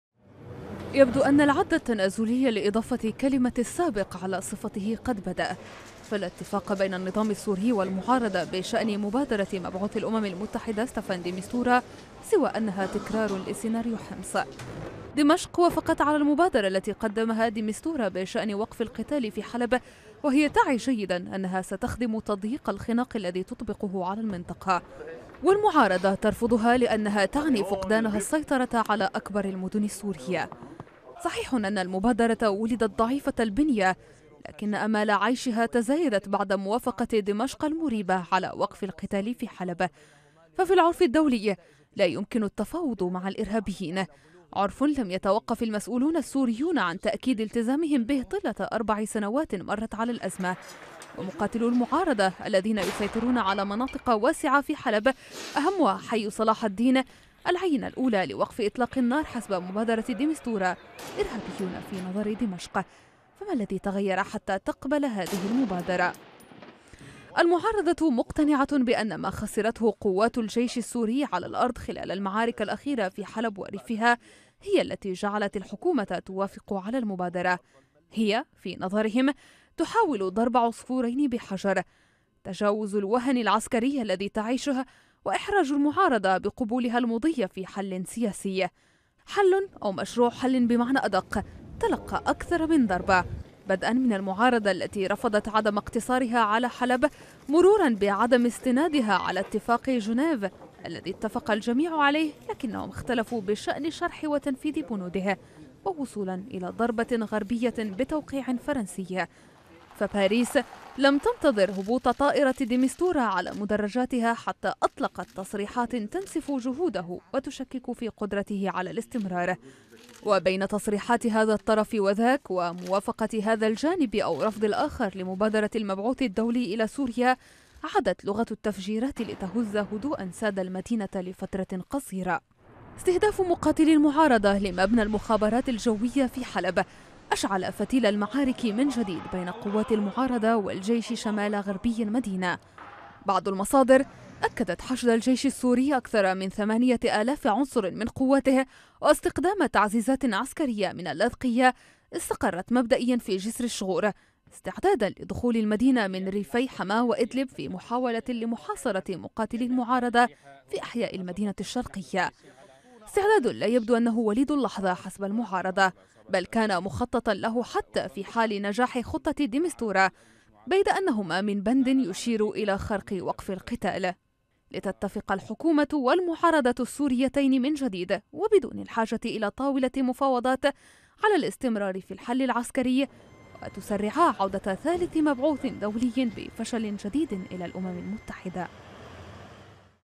حلب بين الحل السياسي والحسم العسكري
فتح التفجير الذي استهدف مبنى المخابرات الجوية في مدينة حلب، النار على مبادرة مبعوث الأمم المتحدة إلى سوريا ستافان ديميستورا، كثيرون يرجحون أن يكون التفجير بمثابة فشل المبادرة الدولية لحل الأزمة في سوريا.